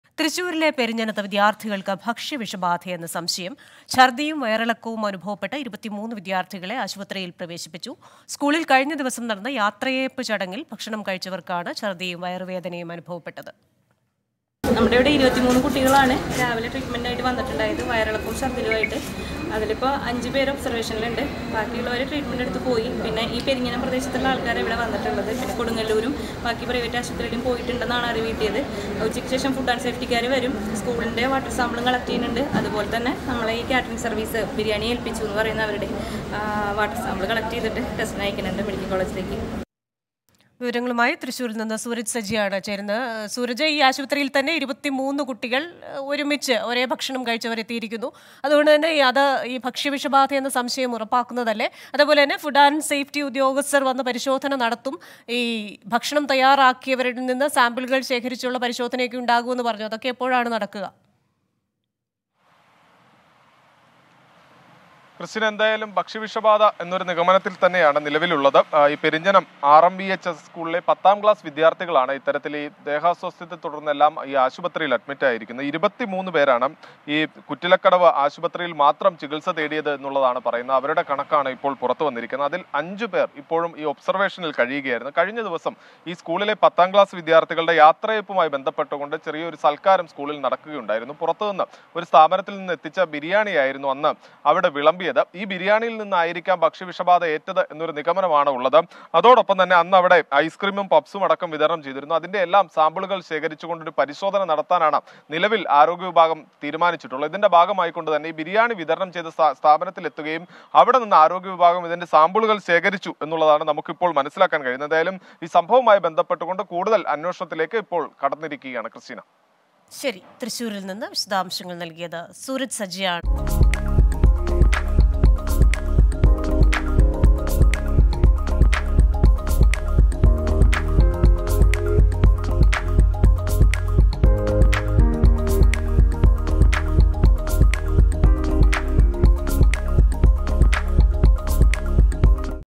തൃശൂരിലെ പെരിഞ്ഞനത്ത് വിദ്യാർത്ഥികൾക്ക് ഭക്ഷ്യവിഷബാധയെന്ന് സംശയം
Suspect food poisoning among students at Perinjanat in Thrissur